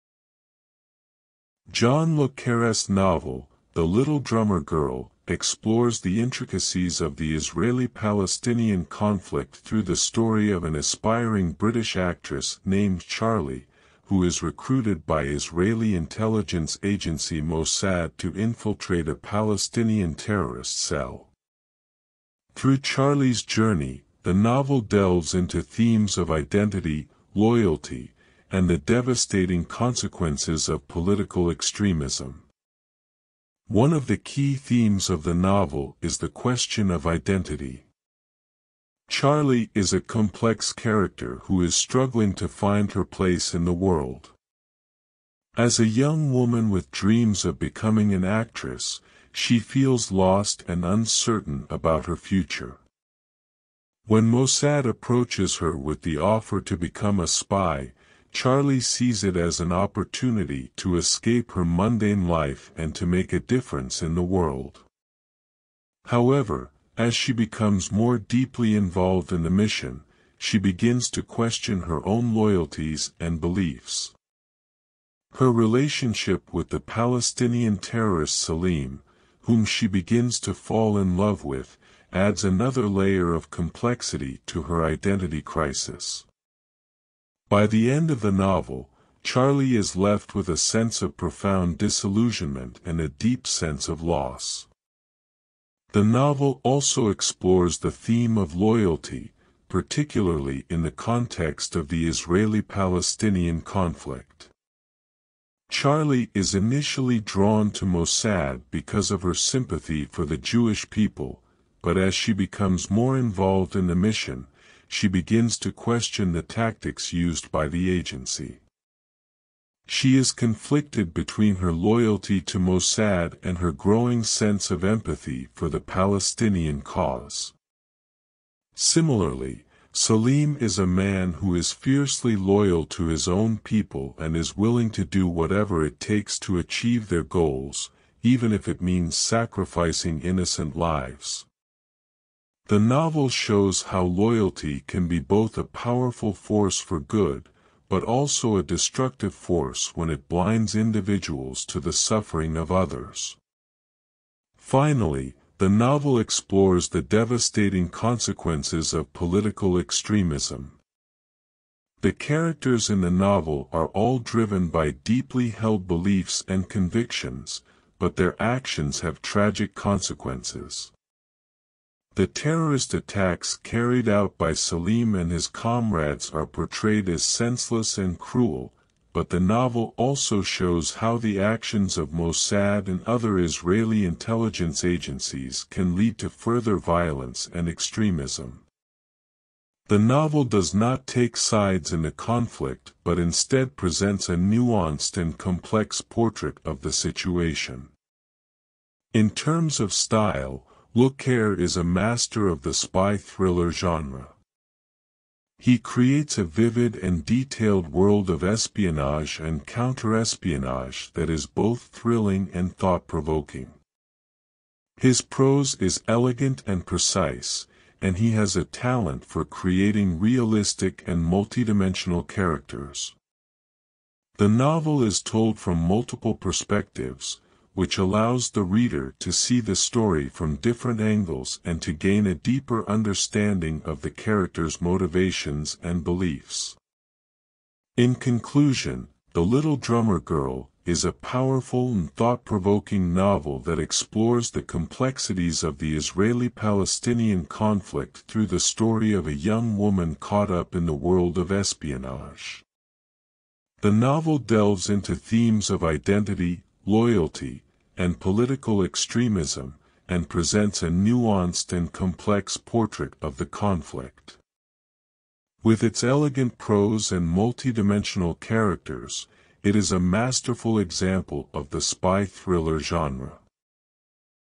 "The Little Drummer Girl" By John le Carré
John le Carré’s novel “The Little Drummer Girl” explores the intricacies of the Israeli-Palestinian conflict through the story of an aspiring British actress named Charlie, who is recruited by Israeli intelligence agency Mossad to infiltrate a Palestinian terrorist cell. Through Charlie’s journey, the novel delves into themes of identity, loyalty, and the devastating consequences of political extremism.

One of the key themes of the novel is the question of identity. Charlie is a complex character who is struggling to find her place in the world. As a young woman with dreams of becoming an actress, she feels lost and uncertain about her future. When Mossad approaches her with the offer to become a spy, Charlie sees it as an opportunity to escape her mundane life and to make a difference in the world. However, as she becomes more deeply involved in the mission, she begins to question her own loyalties and beliefs. Her relationship with the Palestinian terrorist Salim, whom she begins to fall in love with, adds another layer of complexity to her identity crisis. By the end of the novel, Charlie is left with a sense of profound disillusionment and a deep sense of loss.

The novel also explores the theme of loyalty, particularly in the context of the Israeli-Palestinian conflict. Charlie is initially drawn to Mossad because of her sympathy for the Jewish people, but as she becomes more involved in the mission, she begins to question the tactics used by the agency. She is conflicted between her loyalty to Mossad and her growing sense of empathy for the Palestinian cause. Similarly, Salim is a man who is fiercely loyal to his own people and is willing to do whatever it takes to achieve their goals, even if it means sacrificing innocent lives. The novel shows how loyalty can be both a powerful force for good, but also a destructive force when it blinds individuals to the suffering of others.

Finally, the novel explores the devastating consequences of political extremism. The characters in the novel are all driven by deeply held beliefs and convictions, but their actions have tragic consequences. The terrorist attacks carried out by Salim and his comrades are portrayed as senseless and cruel, but the novel also shows how the actions of Mossad and other Israeli intelligence agencies can lead to further violence and extremism. The novel does not take sides in the conflict but instead presents a nuanced and complex portrait of the situation.

In terms of style, le Carré is a master of the spy thriller genre. He creates a vivid and detailed world of espionage and counter-espionage that is both thrilling and thought-provoking. His prose is elegant and precise, and he has a talent for creating realistic and multi-dimensional characters. The novel is told from multiple perspectives, which allows the reader to see the story from different angles and to gain a deeper understanding of the characters’ motivations and beliefs.

In conclusion, “The Little Drummer Girl” is a powerful and thought-provoking novel that explores the complexities of the Israeli-Palestinian conflict through the story of a young woman caught up in the world of espionage. The novel delves into themes of identity, loyalty, and political extremism, and presents a nuanced and complex portrait of the conflict. With its elegant prose and multi-dimensional characters, it is a masterful example of the spy thriller genre.